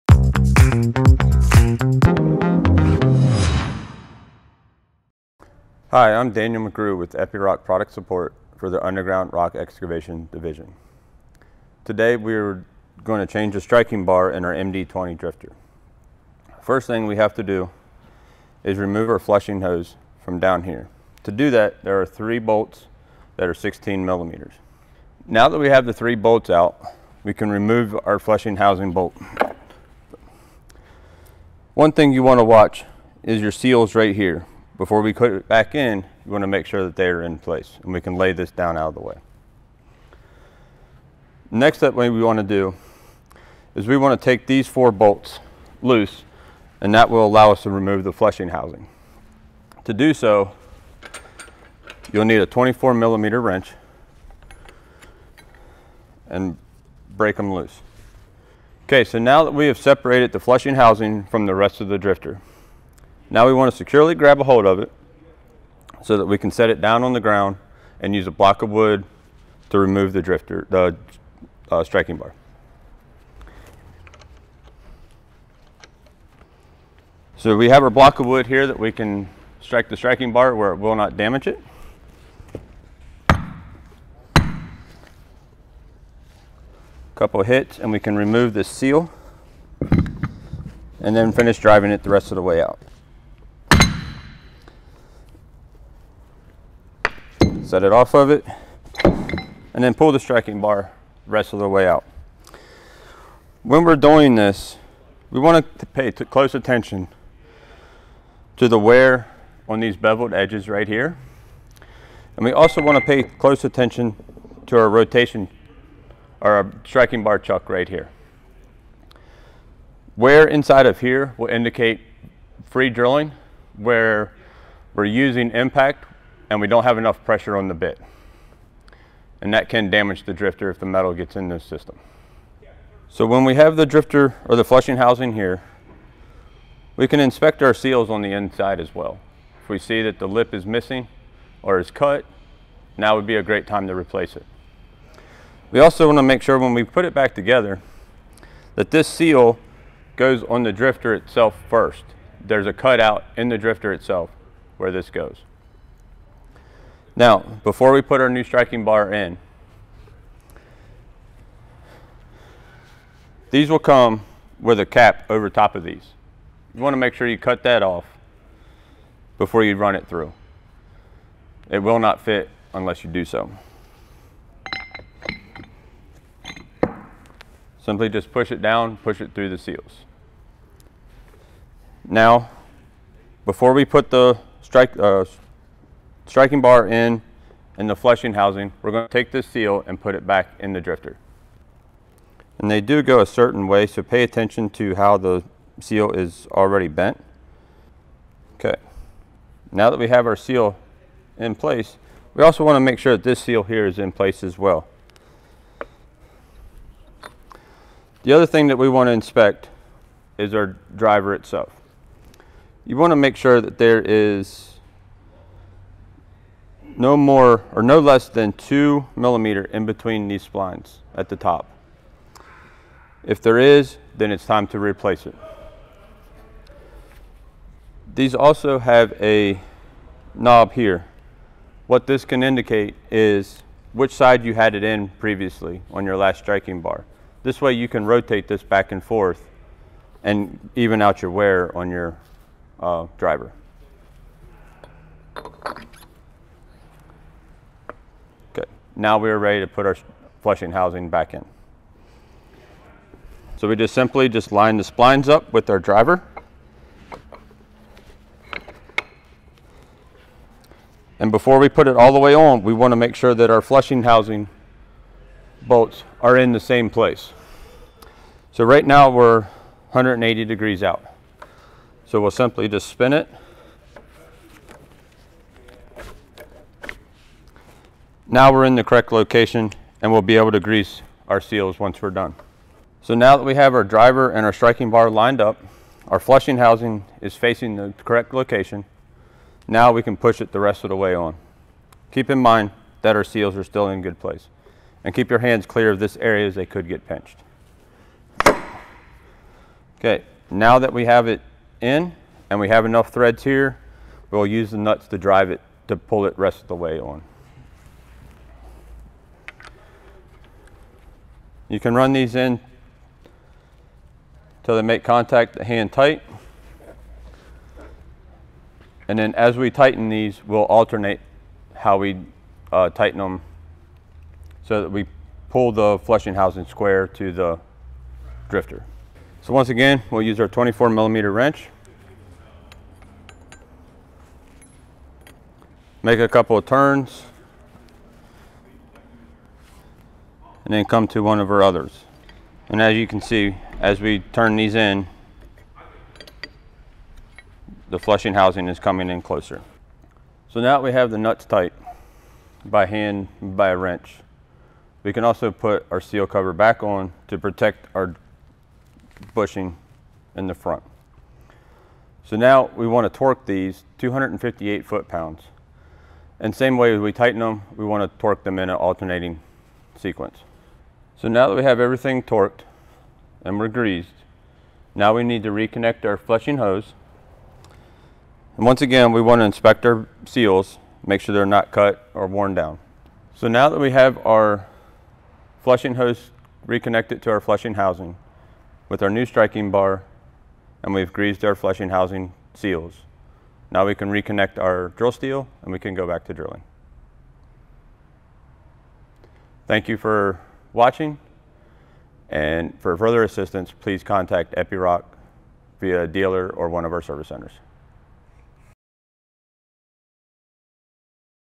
How to change a striking bar | MD20 Drifter
In this Epiroc 24/7 video, Daniel McGrew, Product Support Specialist, demonstrates how to change a striking bar on an MD20 drifter. Learn the step-by-step process to remove the flushing hose, replace the striking bar using a block of wood, inspect seals and wear, align the flushing housing, and reconnect the flushing hose. Improve the performance of your MD20 drifter with this essential maintenance procedure.

Applies to the following models:
MD20 Drifter